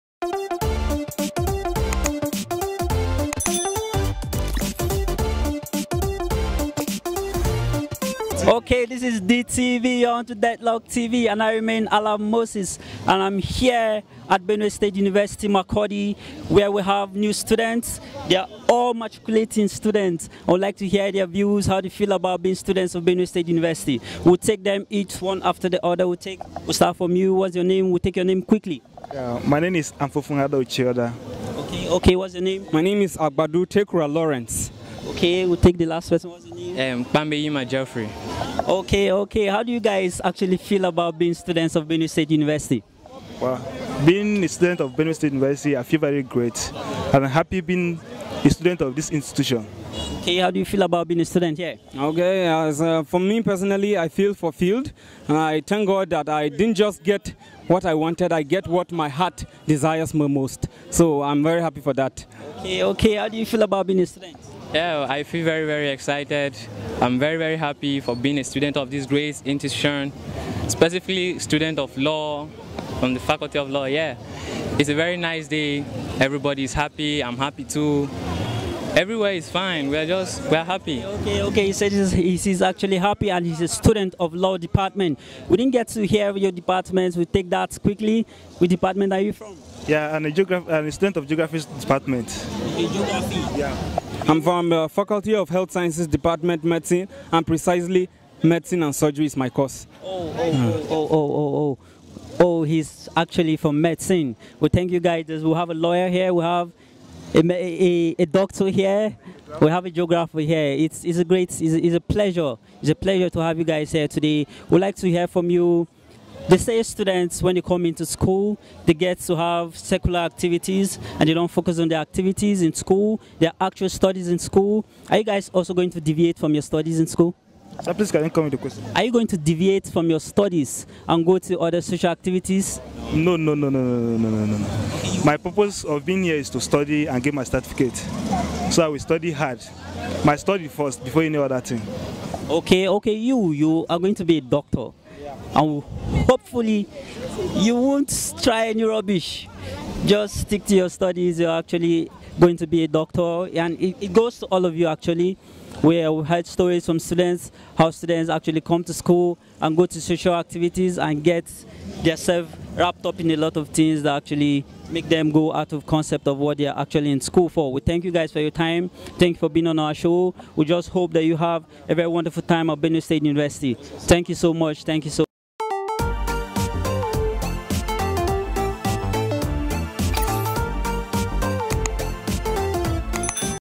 Benue state university matriculation 2018/2019
Benue State Maticulation
The 26th Matriculation Ceremony for fresh undergraduate students for the 2018/2019 academic session.